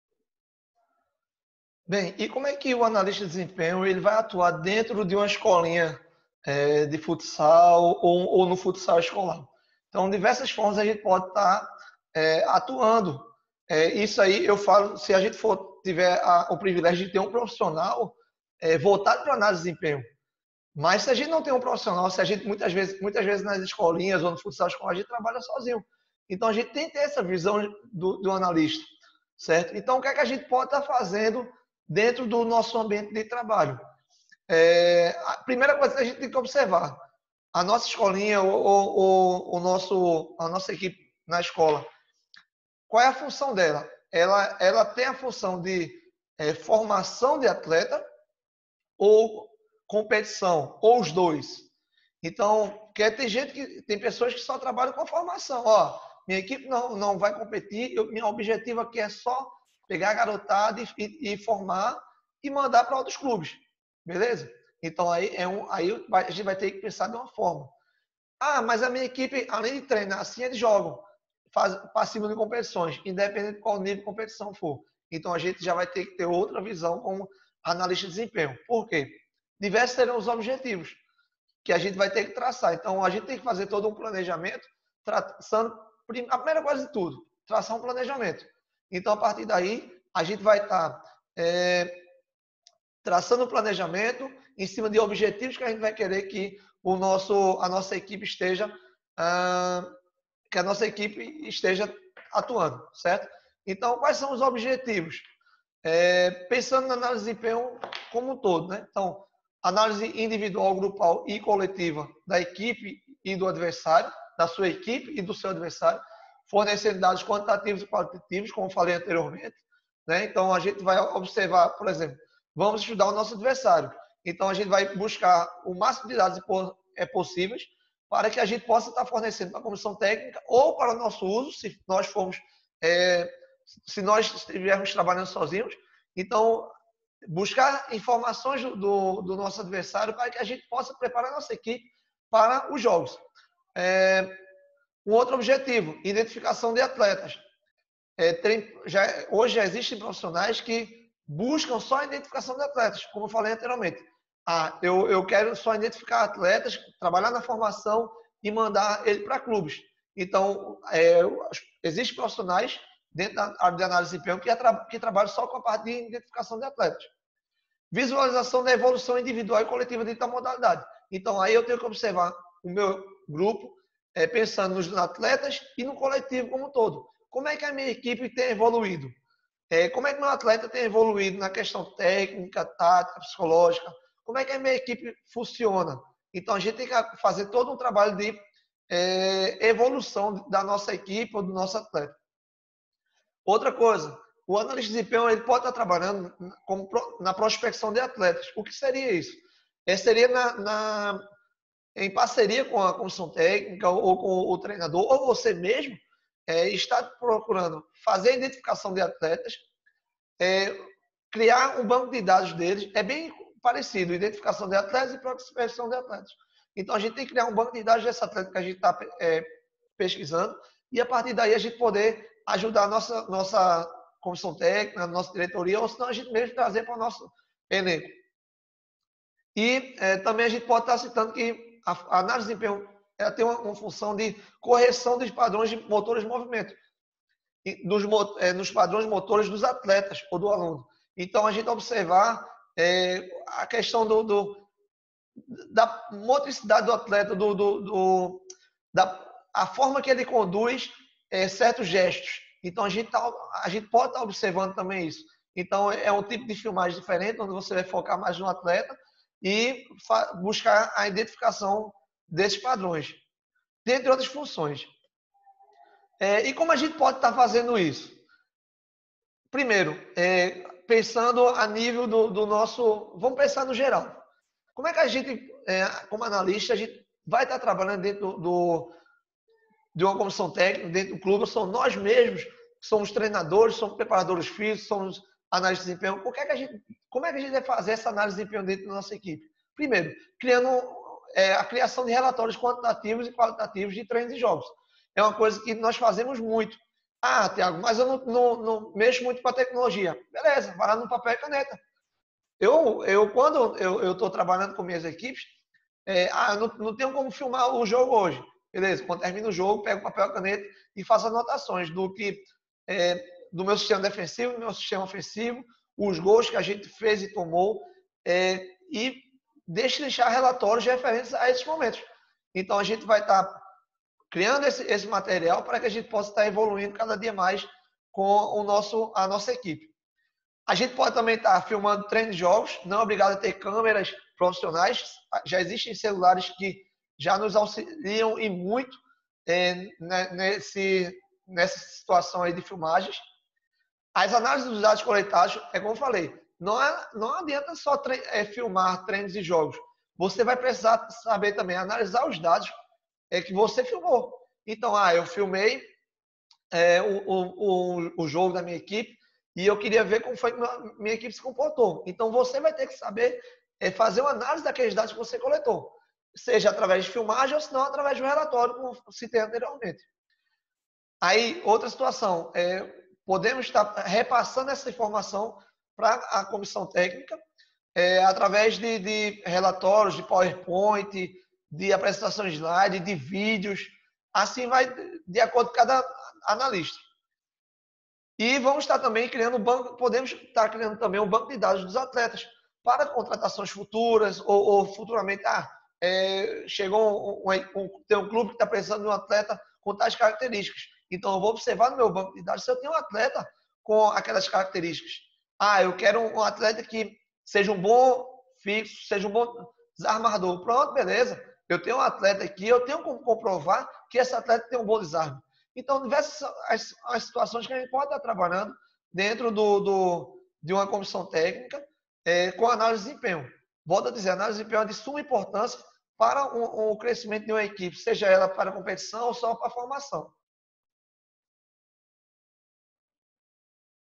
Análise de desempenho no futsal #02 - Thiago Arruda
Conteúdo exclusivo da Conferência Internacional para Treinadores de Futsal
Tema: Análise de desempenho no futsal
Professor: Thiago Arruda